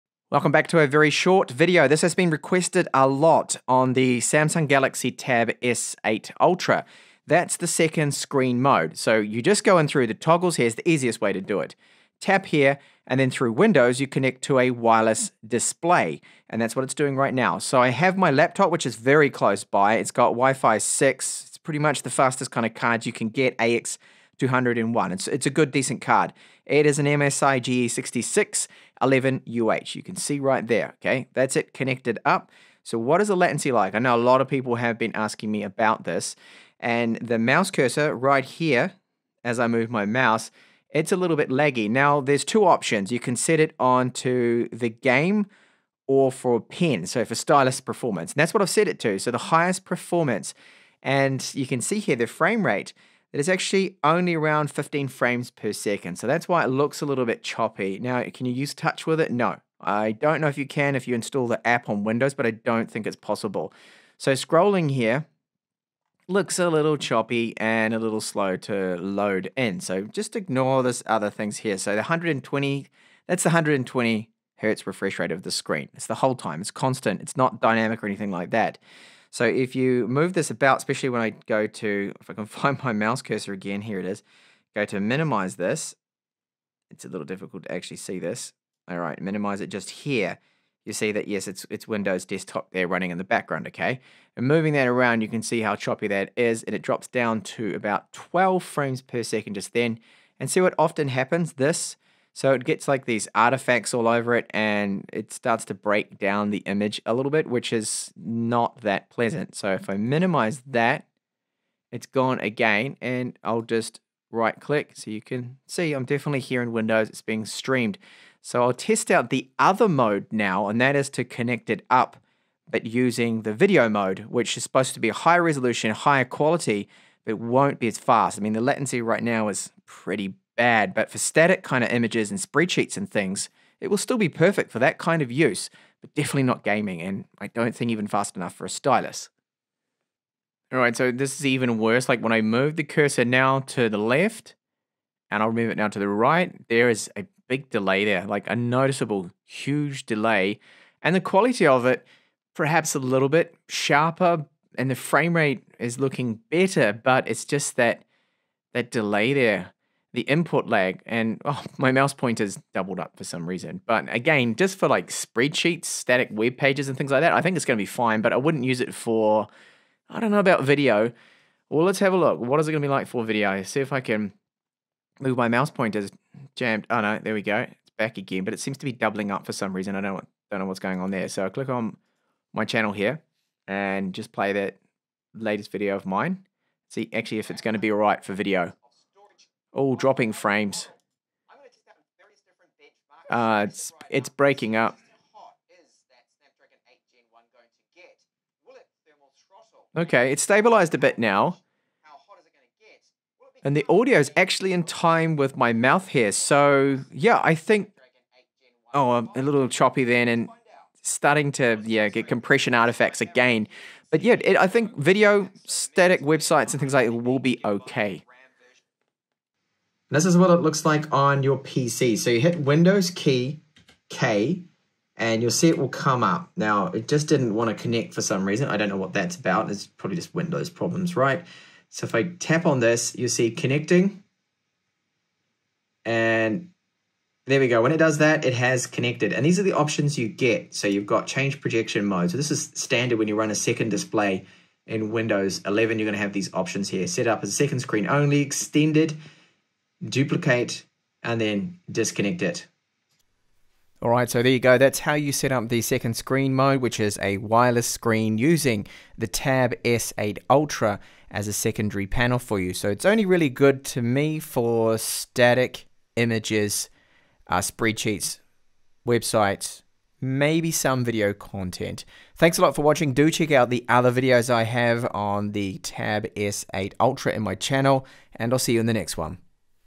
Using My Galaxy Tab S8 Ultra As A Wireless Display (Second Screen Test)
Using the Tab S8 Ultra as a Windows 11 second screen or monitor with the second screen wireless display mode. Latency and FPS test, with video test. It lacks HDMI input which would have been perfect!